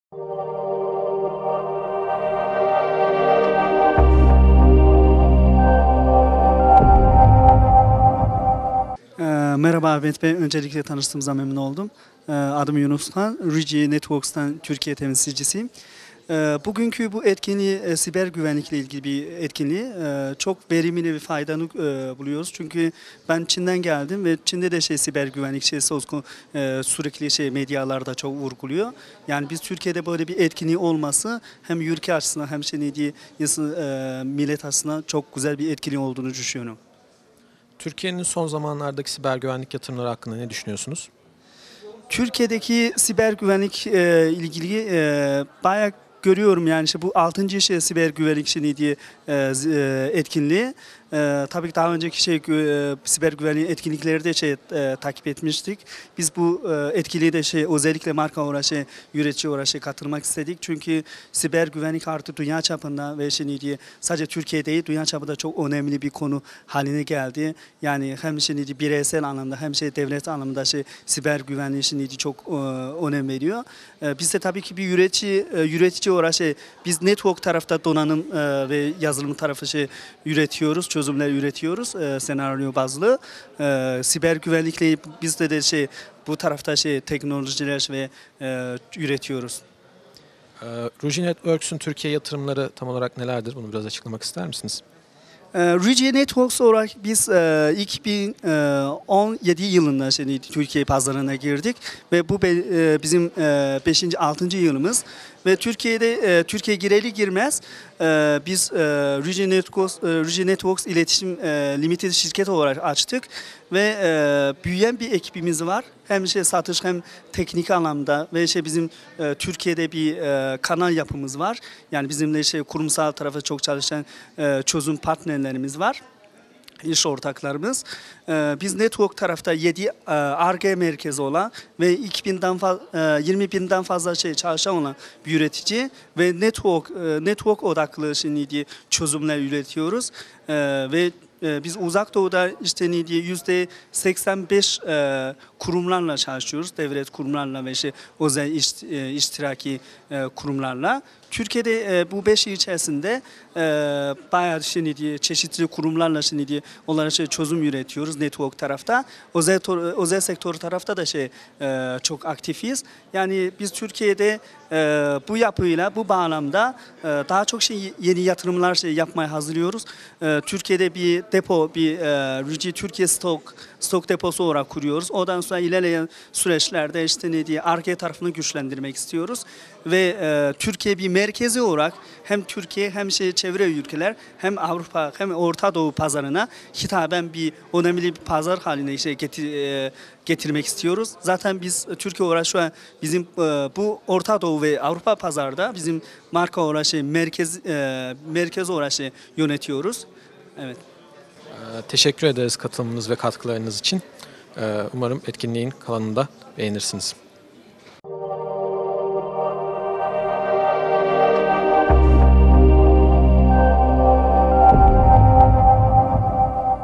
Han: “Verimli ve faydalı bir etkinlik”
17 Mart 2022 tarihinde Bilgi Teknolojileri ve İletişim Kurumu (BTK) binasında gerçekleştirilen 6’ncı e-Safe Siber Güvenlik Zirvesi’nde konuşan Ruijie Networks Türkiye Ülke Müdürü Yunus Han, etkinlik hakkındaki düşüncelerini şu şekilde dile getirdi:

“Siber güvenlikle ilgili bu etkinliği çok verimli ve faydalı buluyorum. Türkiye’de böyle bir etkinliğin olması hem ülke açısından hem de millet açısından çok güzel."

Türkiye’nin siber güvenlik yatırımlarına ilişkinde konuşan Han, “Siber güvenlik sadece Türkiye değil, dünya çapında da çok önemli bir konu haline geldi. Hem bireysel hem de devlet anlamında siber güvenliğe çok önem veriliyor. Biz de bir üretici olarak network tarafında çözümler üretiyoruz” dedi.

VİDEO: 648
BAŞLIK: Han: “Verimli ve faydalı bir etkinlik”
KONUŞMACI: Yunus Han – Ruijie Networks Türkiye Ülke Müdürü
ETKİNLİK: 6.e-Safe Siber Güvenlik Zirvesi
TARİH: 17 Mart 2022, Perşembe